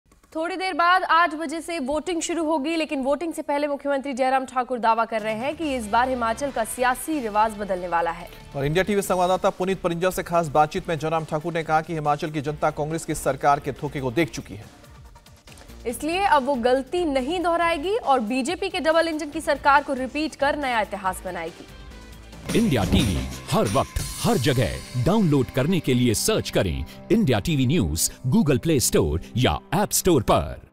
CM Jairam Thakur claims BJP to win Himachal elections with majority votes | Himachal election
More than 55 lakh voters will vote the 68 assembly segments in Himachal Pradesh today. At the nub of the fight is whether the BJP succumbs to the Himalayan state's "rivaaj", or tradition, of changing the government. Results will be out on December 8.